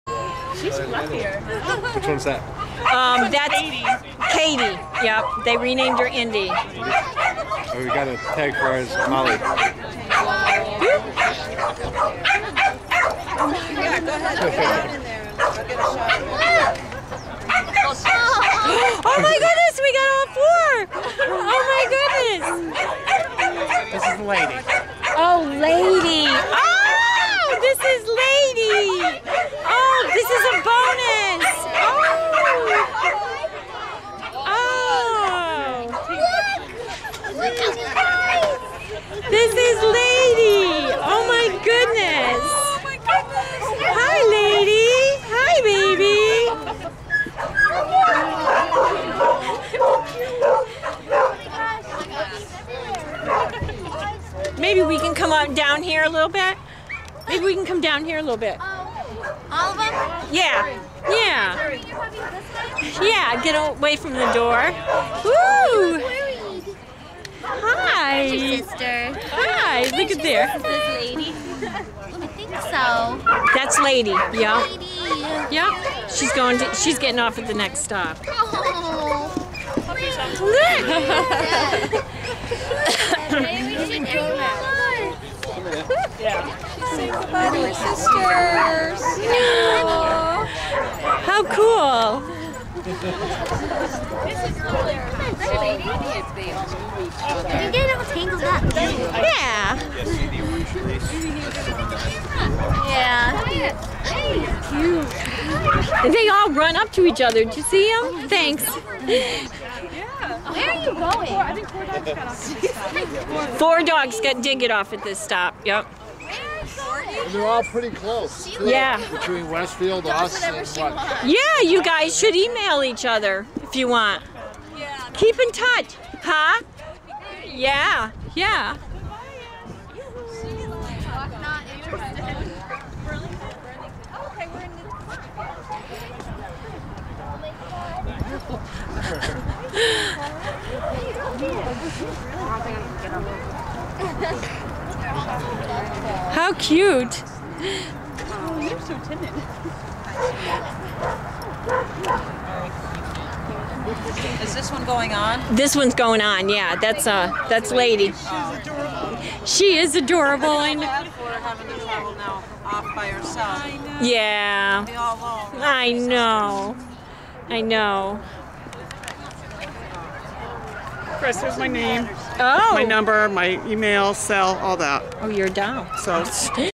SISTERS MEET.mpg
All adopters agree to meet again and get the sisters together in the near future.